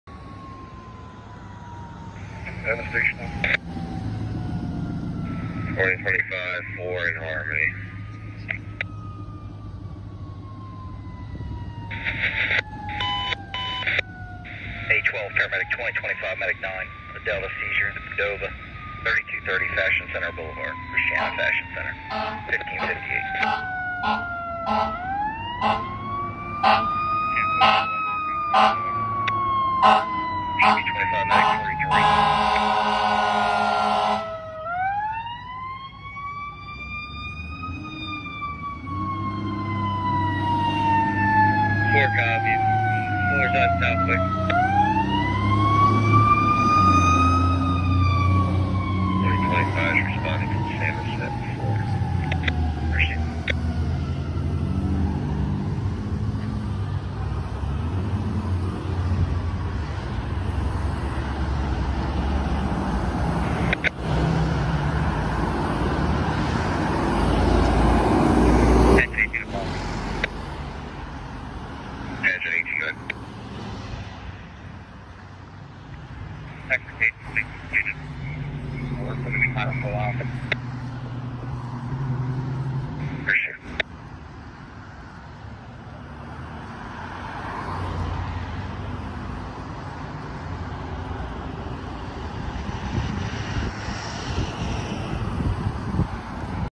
HAZMAT30 responding to assist the City of Wilmington with a 10-10 w/ a Large Fuel Spill
(Response recorded on 10/19/2021)